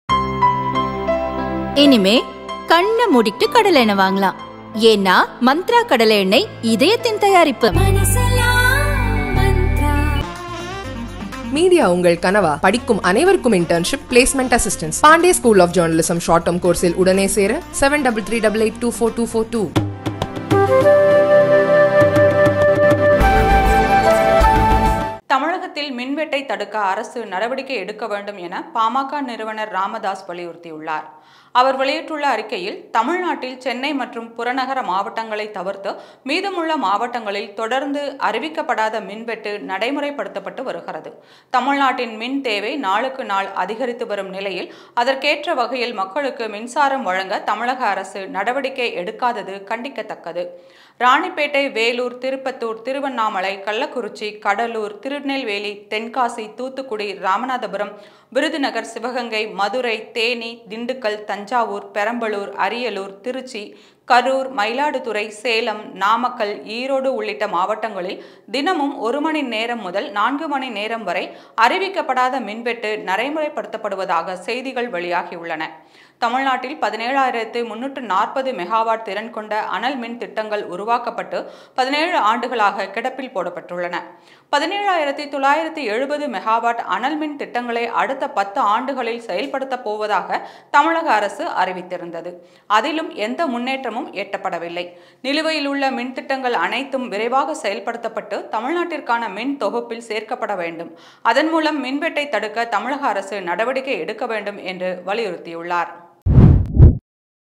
அறிவிக்கப்படாத மின்வெட்டு?? ராமதாஸ் கண்டனம் | Powercut | DMK | Ramadoss | PMK | Tamilnadu Goverment
சாணக்யா!